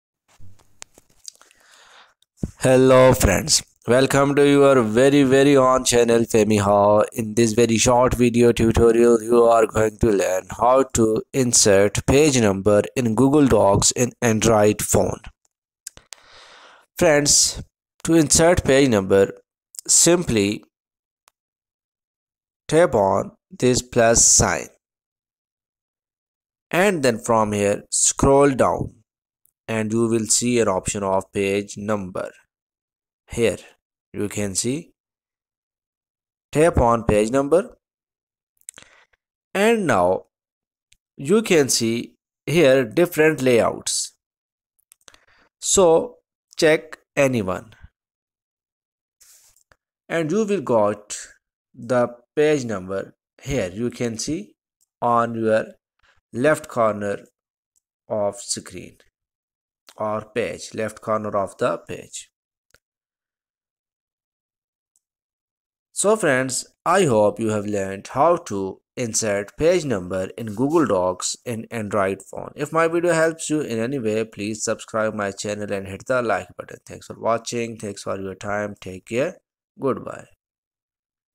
How to Insert Page Number in Google Docs in Mobile Phone | How to Add Page Number in Google Docs
This Video Tutorial Will show you the way to insert page number in Google Docs in Mobile Phone. after watching this video tutorial you will be able to add page number in Google Docs in Android Phone.
This Video Tutorial may work in Google Docs in All Android Phones.